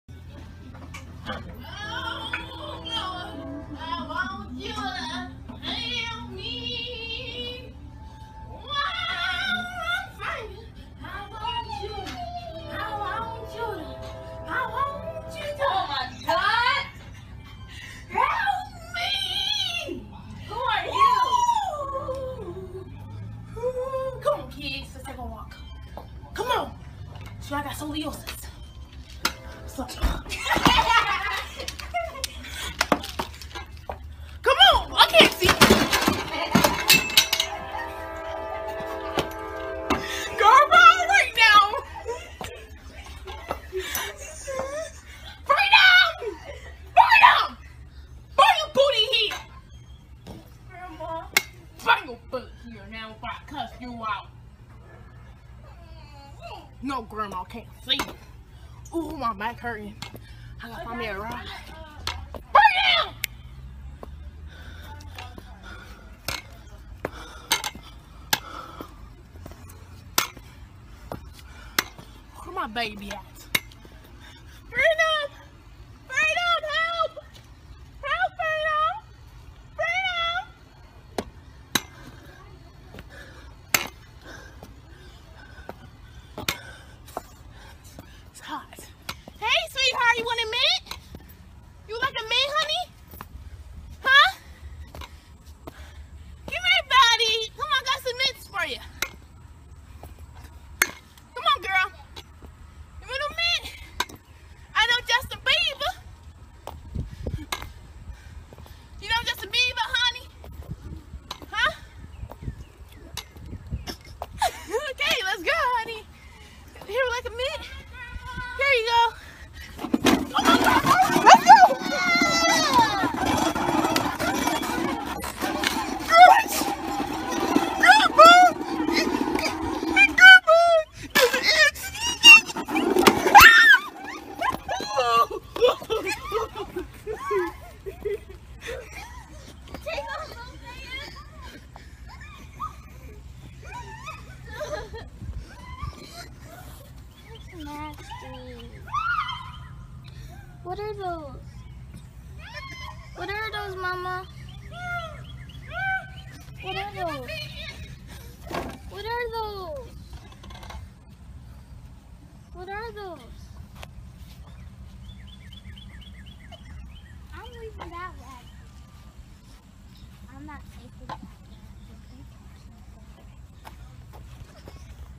Grandma Margo falls hard
Grandma Margo rides in a wagon full of dead magnets